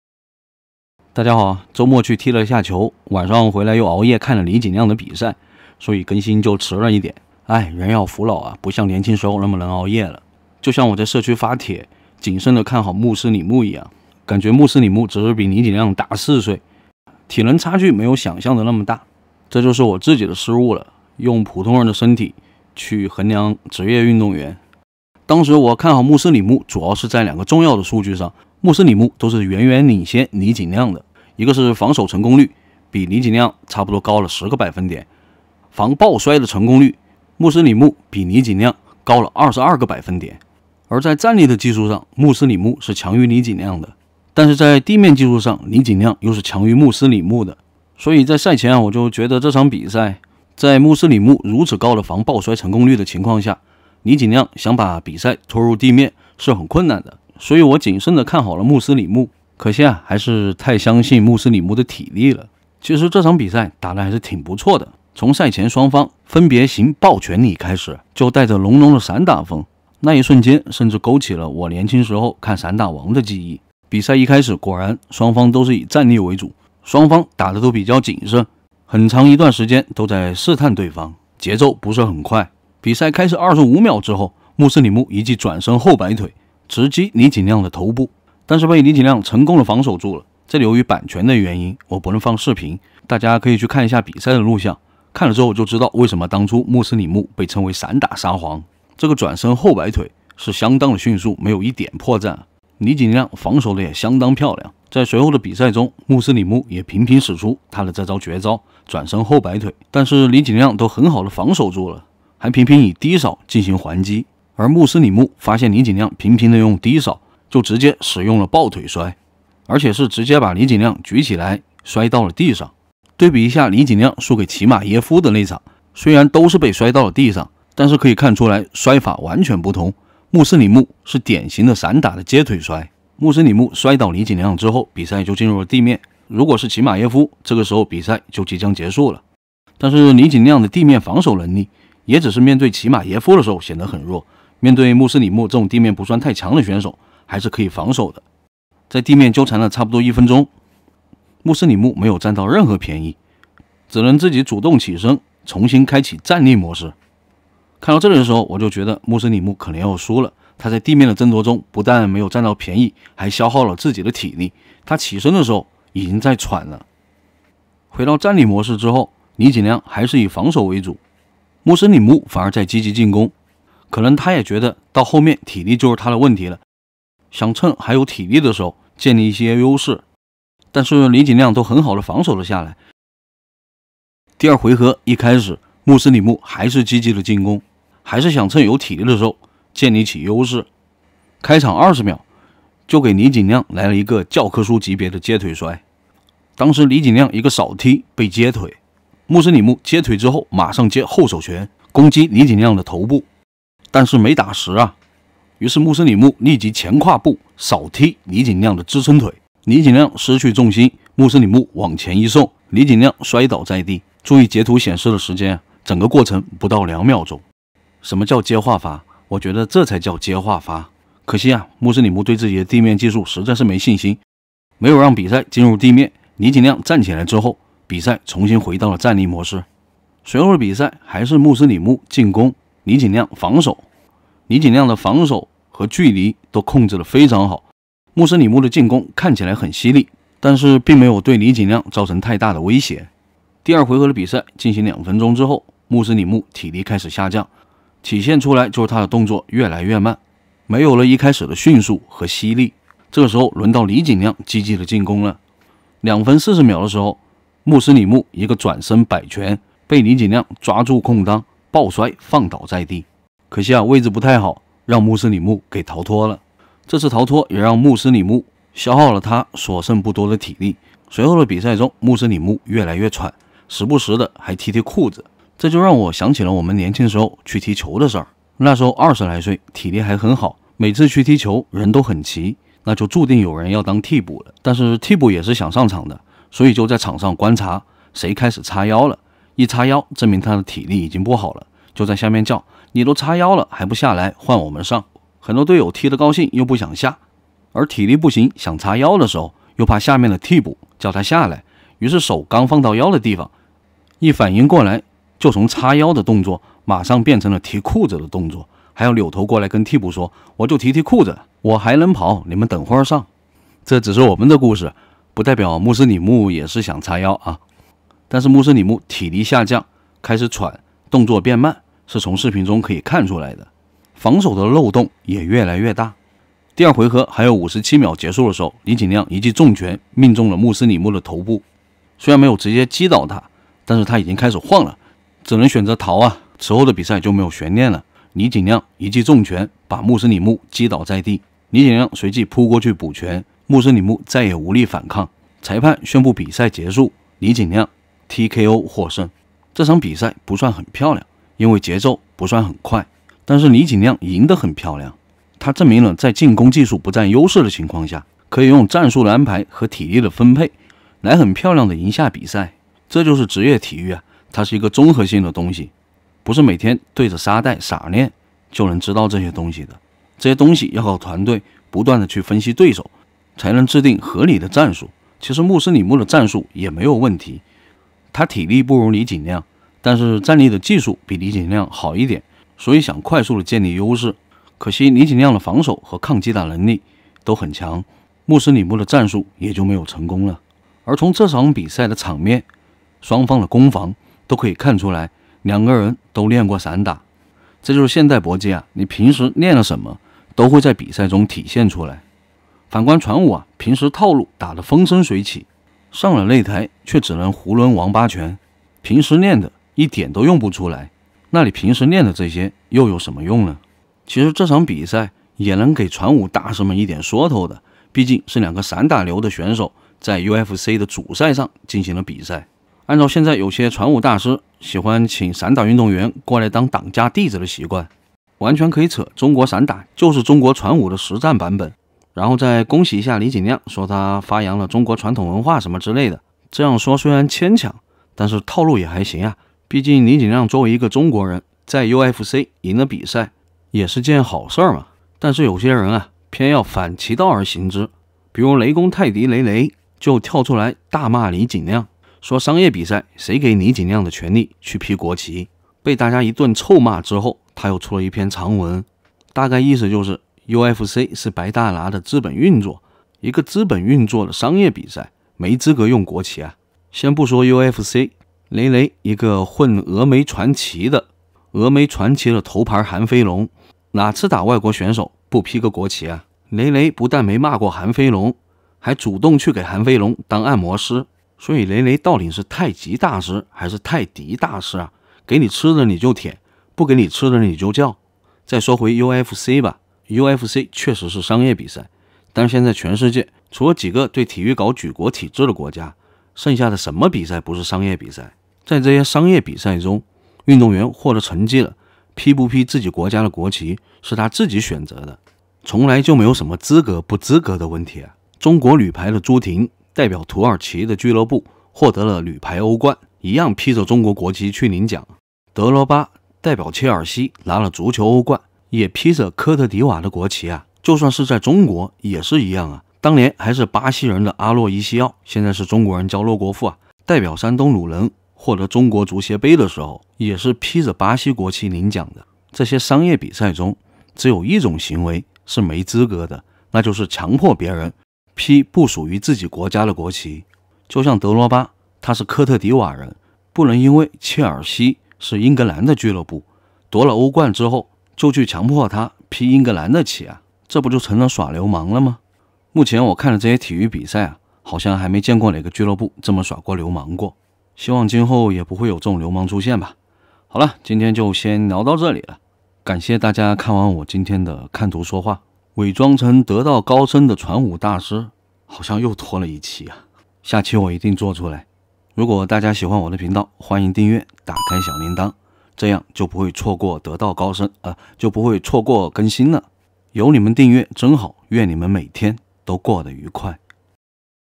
李景亮TKO穆斯里穆後雷雷狂噴：都是資本運作，李景亮，你也配？
歡迎成為我的YT頻道會員